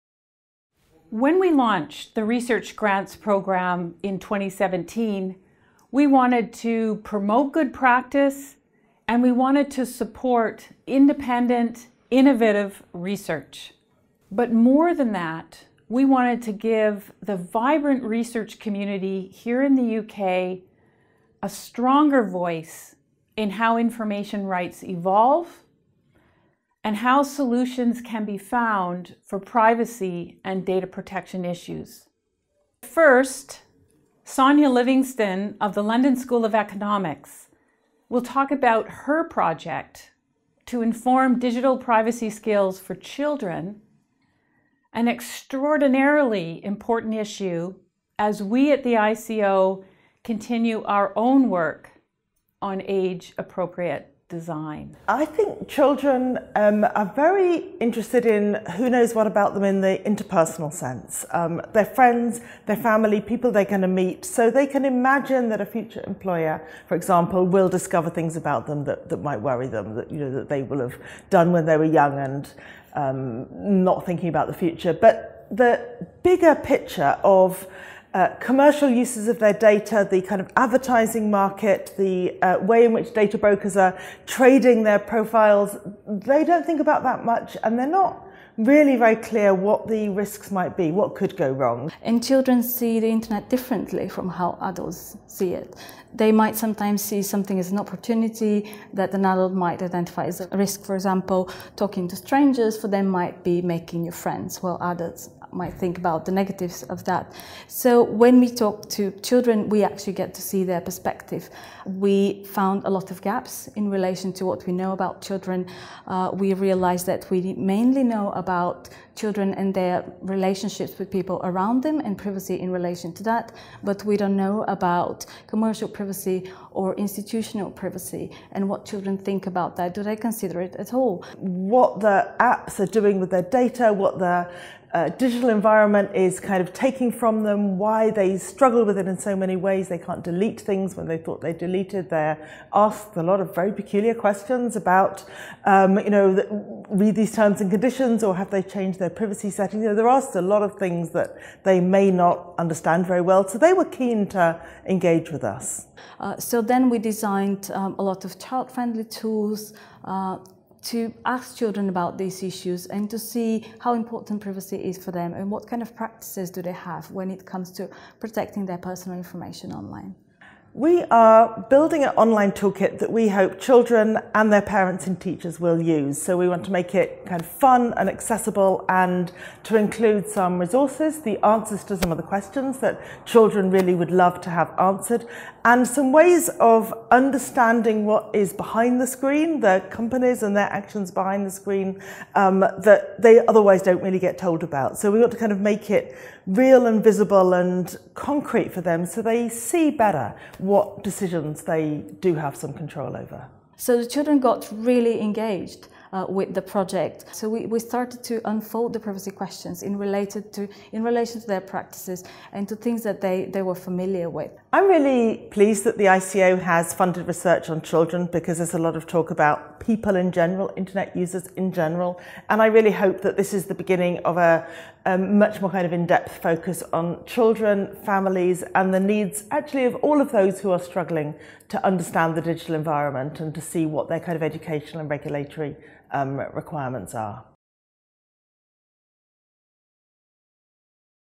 Children's Data and Privacy Online: Information Commissioner's Office Grants Programme
Prof Sonia Livingstone and Dr Mariya Stoilova talk about their project Children's Data and Privacy Online: Growing up in a digital age, funded by the Information Commissioner's Office (ICO).

In a datafied age, it is becoming pressing that children grasp the ways in which all the information they give online, and all the sites they visit or ‘likes’ and ‘comments’ they make, leave a trace that can be collected, kept and combined with other data to build a profile of them which advertisers will pay for. Data protection regulation protects users’ privacy rights, but how do children think about their privacy, and what decisions do they make?

The Children's Data and Privacy Online project seeks to address questions and evidence gaps concerning children’s conception of privacy online, their capacity to consent, their functional skills (e.g. in understanding terms and conditions or managing privacy settings online), and their deeper critical understanding of the online environment, including both its interpersonal and, especially, its commercial dimensions (including its business models, uses of data and algorithms, forms of redress, commercial interests, systems of trust and governance).